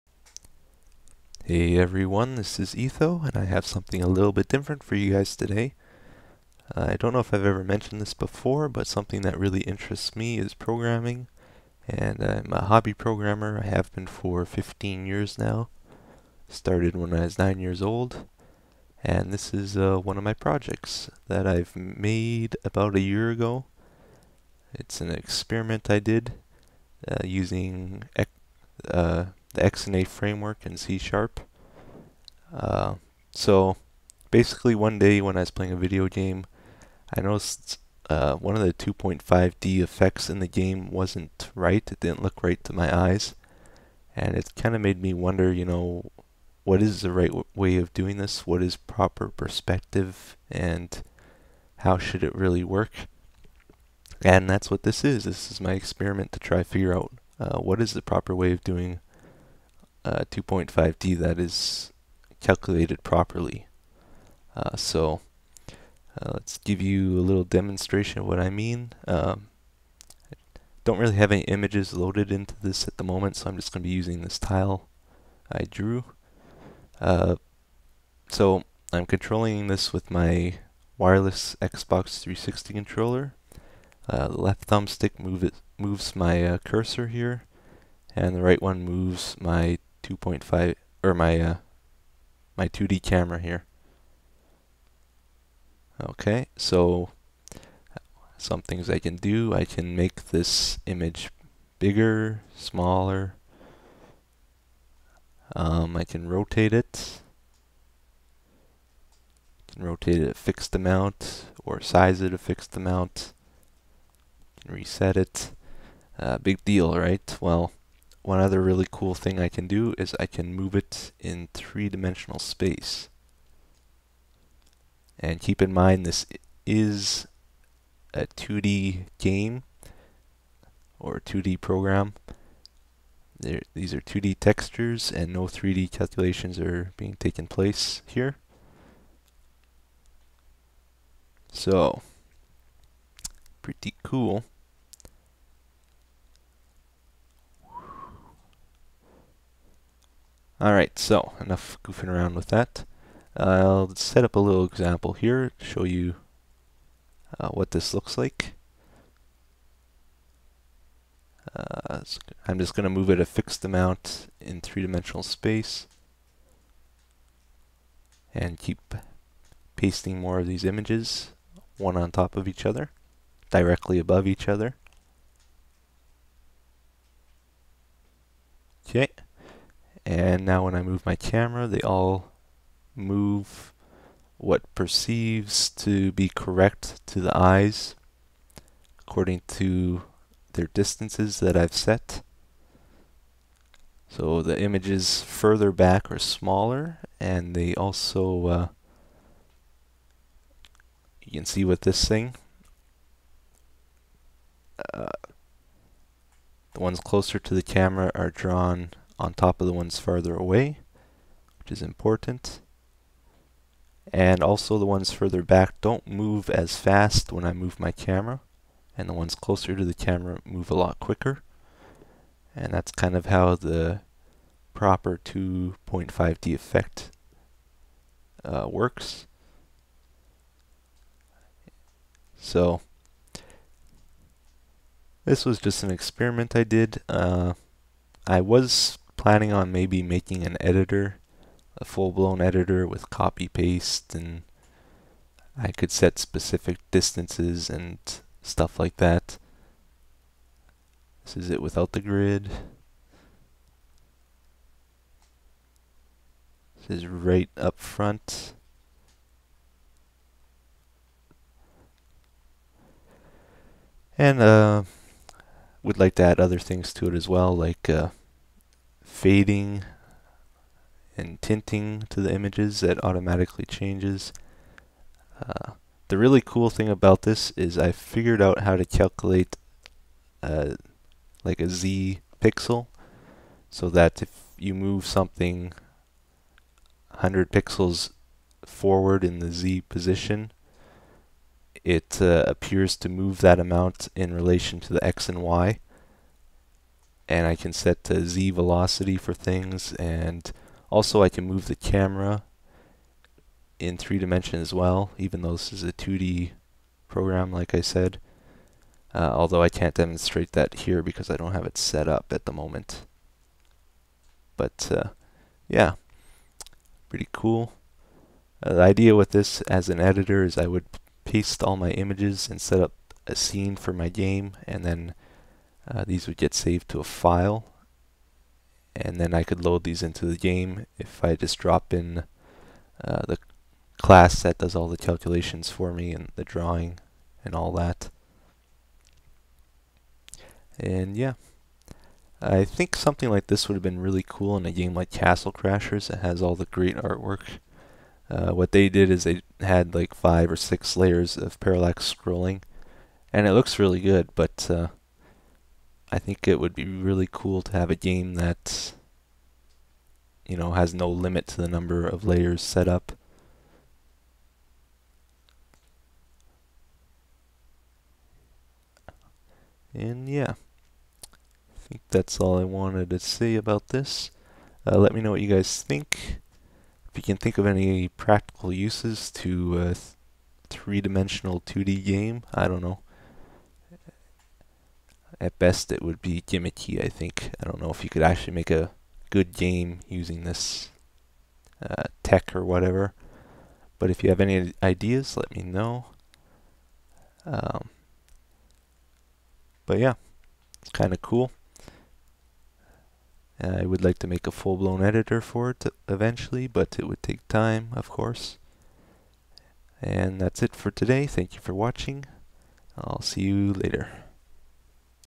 Tech: Experimental 2.5D Engine
I just wanted to show off an experimental project I did. This was an attempt to figure out how to calculate proper perspective for a 2.5D engine.

Most games just setup 1 or more layers of parallax scrolling in the background to give the game a feeling of depth. Poorly done artwork or poorly chosen scrolling speeds for the background layers can make a game look strange.

One day I would like to fully develop this idea and make sophisticated editor for it.